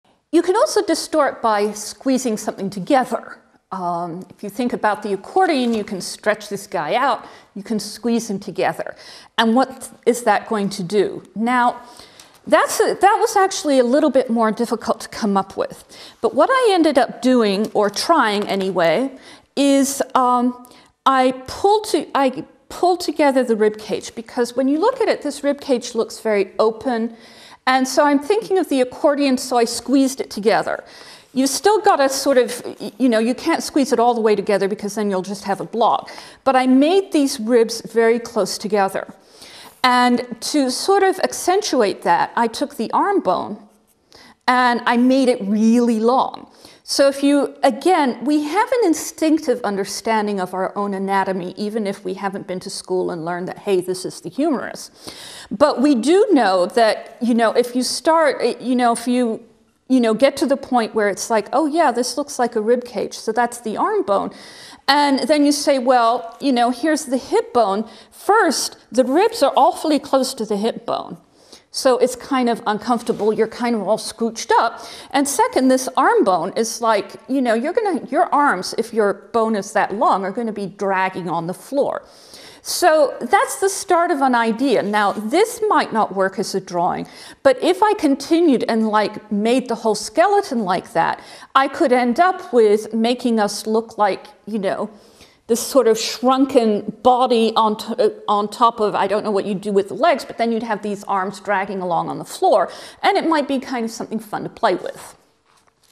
Creating Abstract Art: Design Principles : Creating Abstract Art: Squeezing a Subject
By squeezing familiar shapes and figures, abstract artists can come up with new designs. Distort subjects in abstract painting by using the techniques in this free art lesson from an art instructor.

Expert: Gretchen Kibbe
Bio: Gretchen Kibbe is an artist and part-time faculty member at Appalachian State University. She worked as a scenic artist on the Spike Lee movie School Daze.